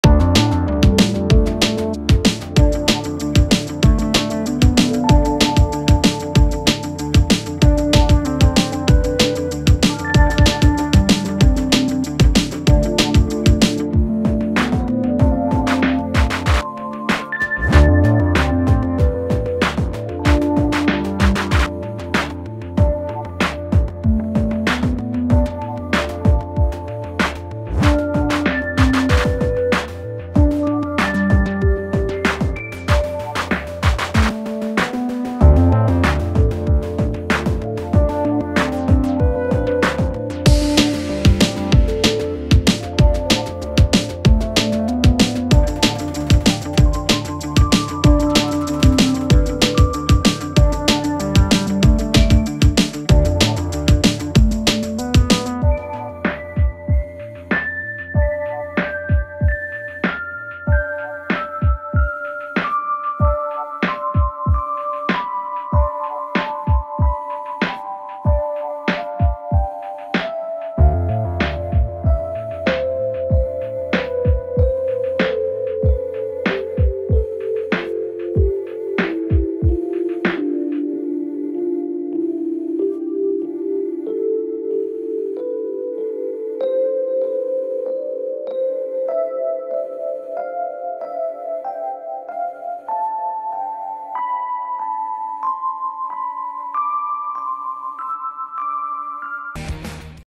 Nissan All New Livina lampu terang dan keren functional Part 3
terima kasih utk konsumen yg percaya dengan produk yg berkwalitas pasti tau produk lampu yg tepat dan terbaik, upgrade kunjungan pertama kali datang dari halogen yg ingin lebih terang ke led

upgrade kunjungan ketiga yg ingin lebih terang lagi dan lebih gaya biar gk kalah sama mobil2 yg udah pakai projector dan lebih functional dipasangkan projector full spek
autovisionlighting dr bowlnya sampai dalaman hid nya... hasilnya lebih maksimal

berminat silahkan bisa datang langsung ke lokasi workshop Jakarta Pusat,
MGK Kemayoran Lt. 6, Blok A.19, No.1, WA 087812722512